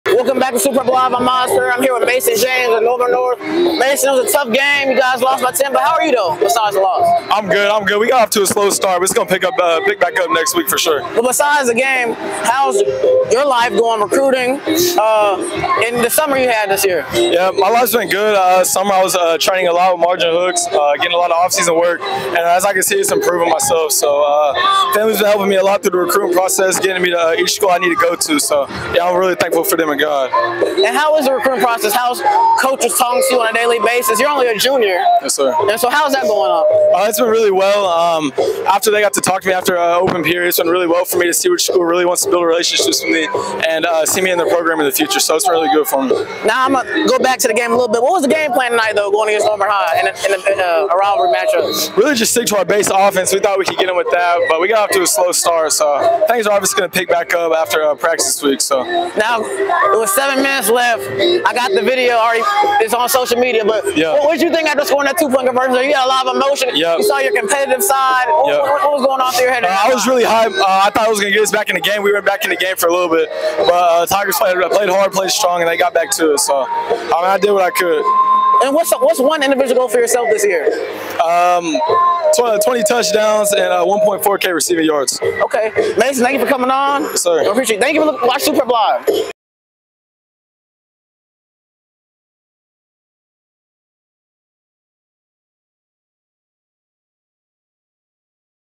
Super Prep’s Miles Perry talks with Norman North 4🌟 Mason James ‘26
Super Prep’s Miles Perry talks with Norman North 4🌟 Mason James ‘26 (WR) about the Norman High rivalry, recruiting, and more.

James had four catches for 106 yards against Norman High.

He holds offers from Oklahoma, Michigan, Alabama, Ole Miss, Tennessee, and more.